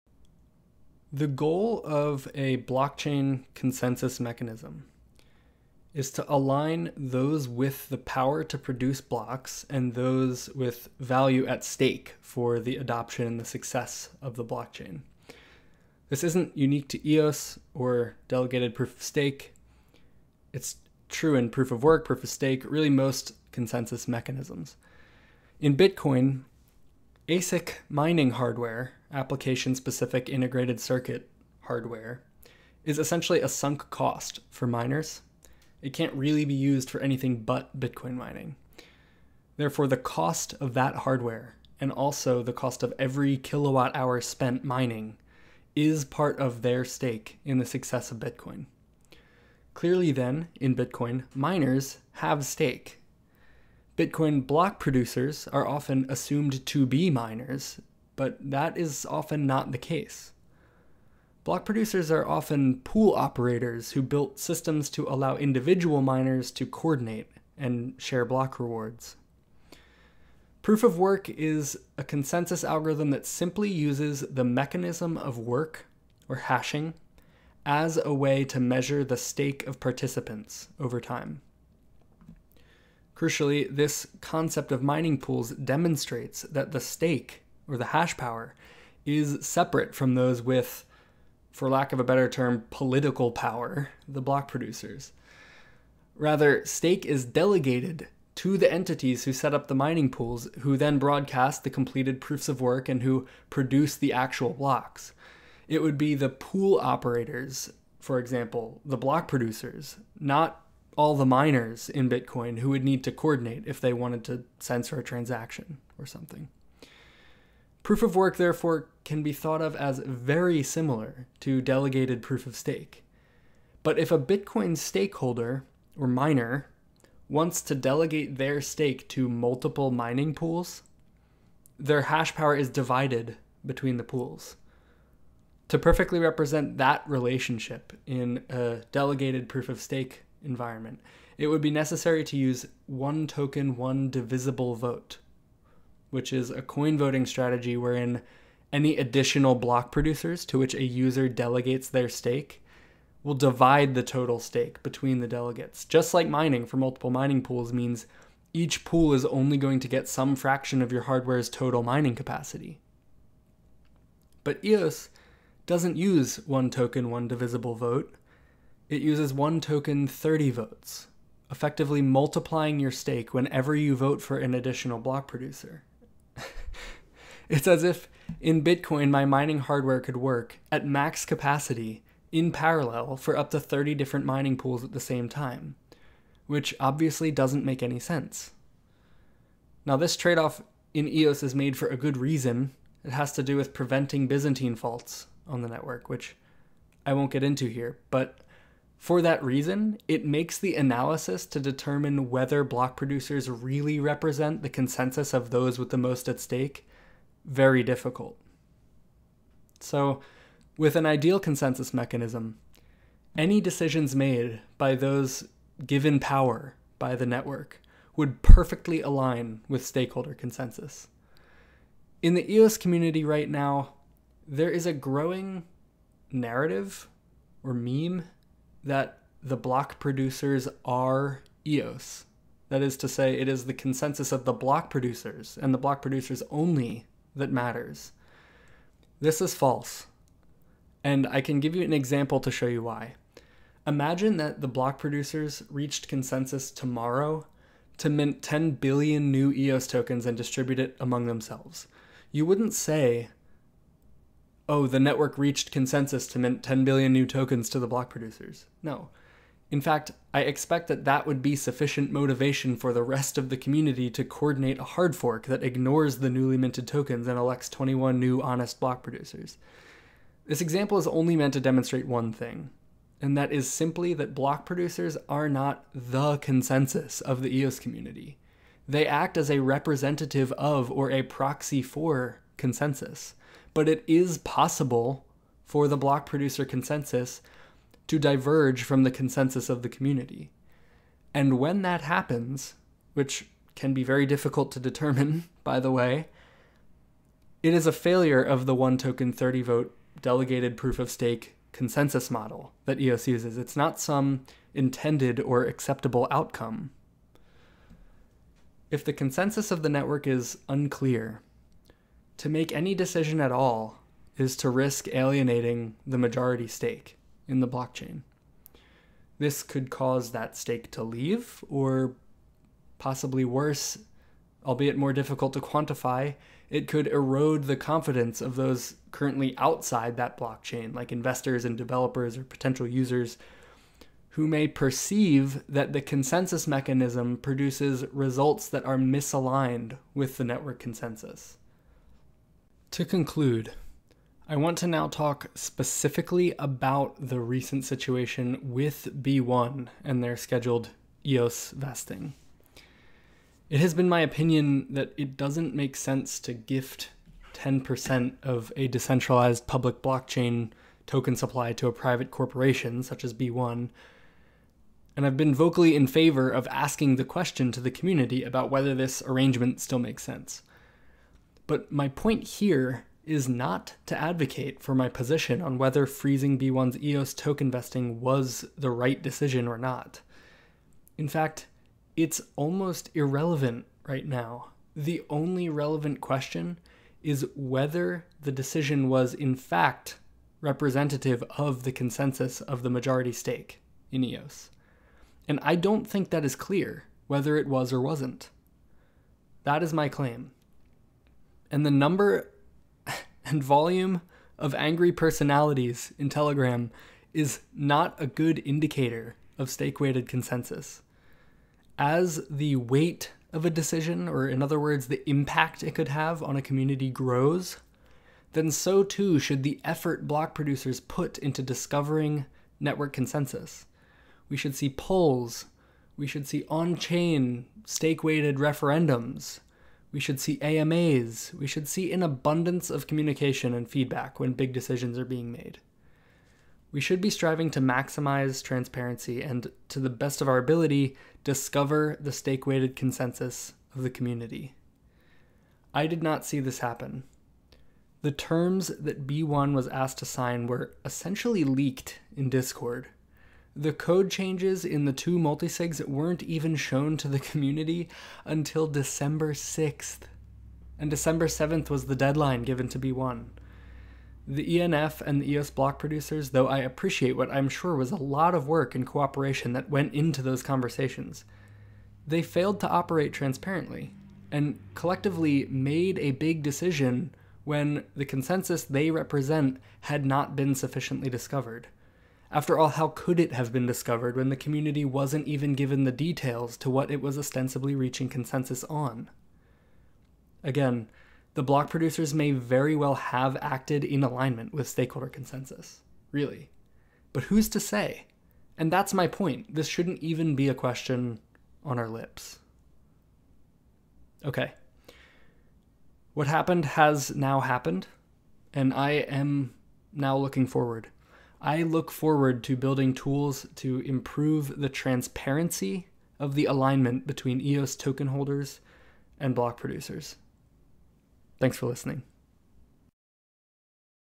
ENF/B1 Negotiations - Retrospective
Audio only, sorry folks. I start out discussing the fundamental nature of "stake" on a blockchain and how it relates to those who produce blocks.

At the end I specifically discuss the recent B1 & ENF negotiations that culminated in the pausing of the network's payment to B1 (vesting 10% EOS supply over 10 years).